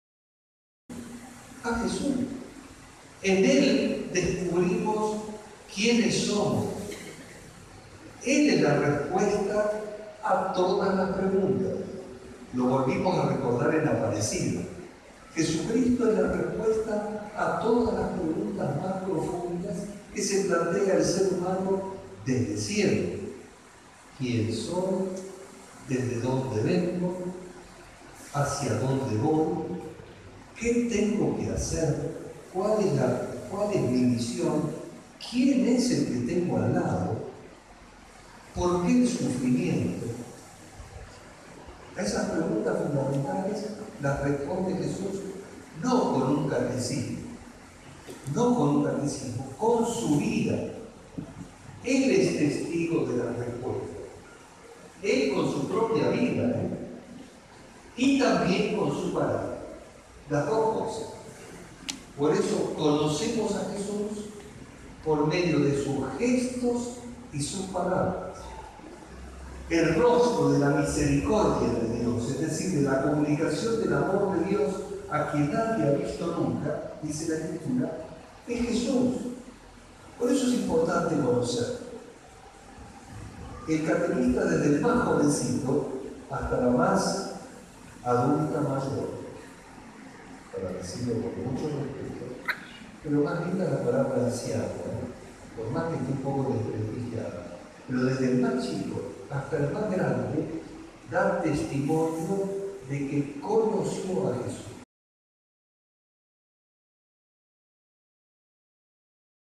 Encuentro Arquidocesano de Catequistas
Se lleva a cabo este domingo el Encuentro Arquidocesano de Catequistas en la localidad de Saladas. Monseñor Stanovnik presidió la bendición y dio una disertación.